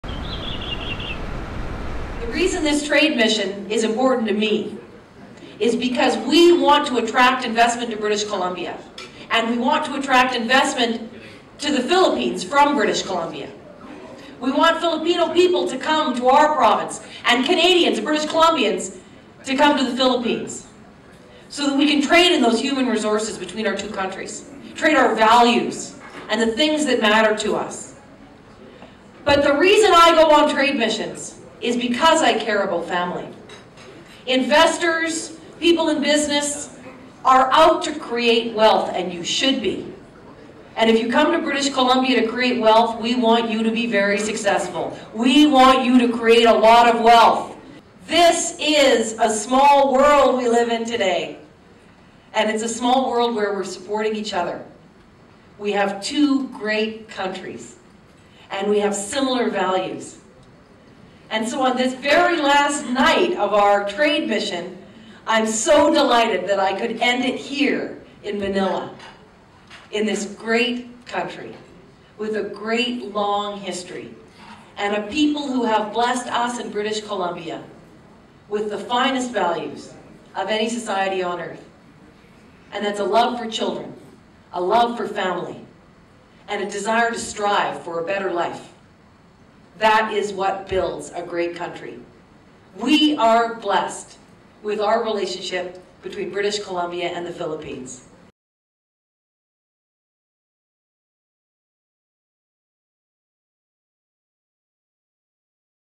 Labour, Economic Opportunity Focus of Philippines Visit
On the final day of this Jobs and Trade Mission, Premier Christy Clark strengthened the relationship with the Philippines by signing a Joint Communiqué on labour mobility and highlighting the tremendous opportunities and economic partnerships available in British Columbia, such as the new, made-in-B.C. clean-energy facility.